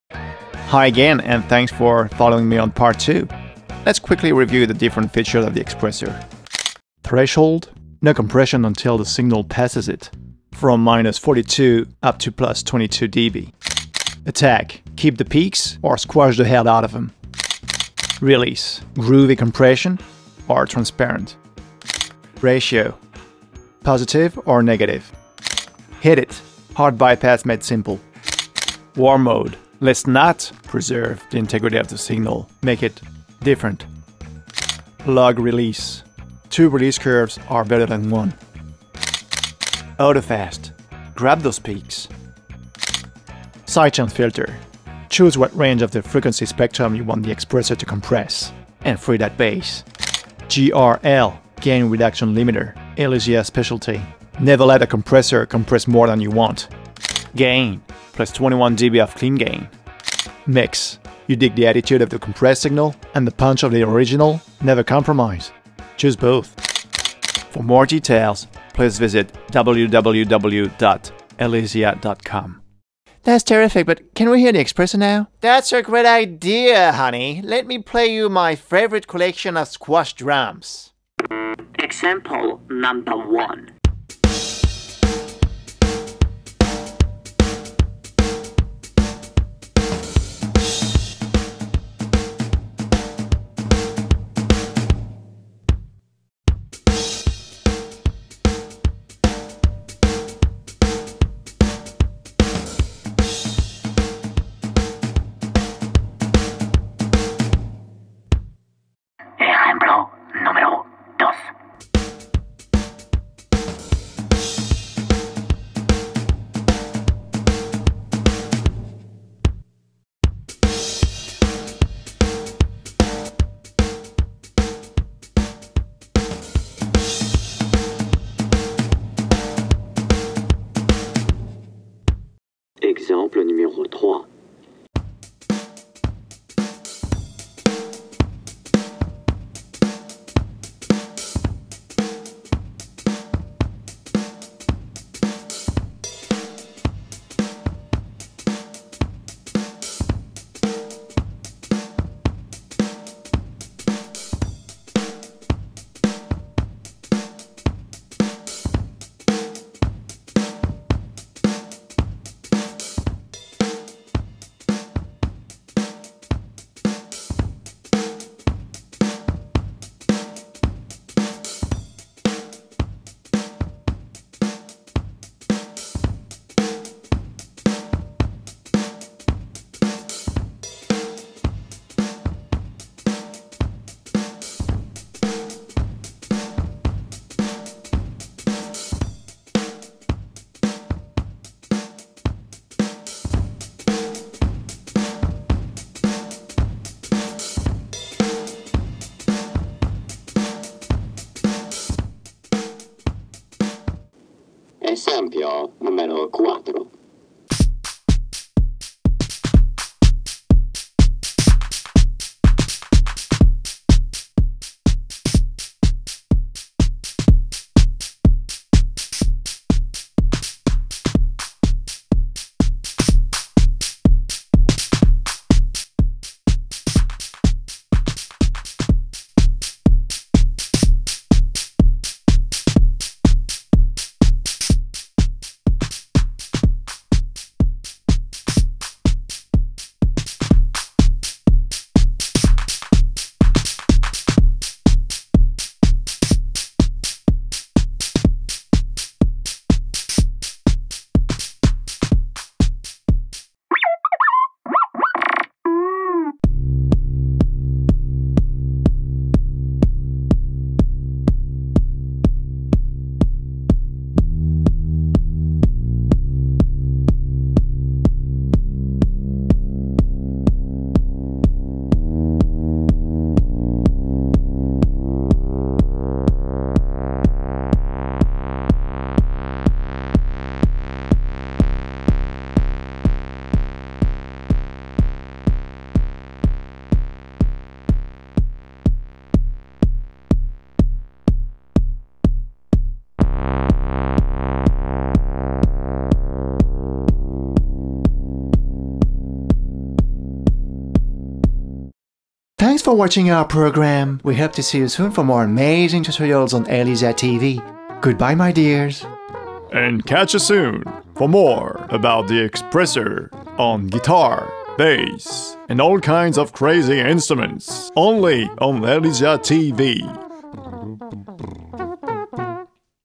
xpressor workout part 2
Welcome back!
Lots of nice xpressor compression examples are waiting for you...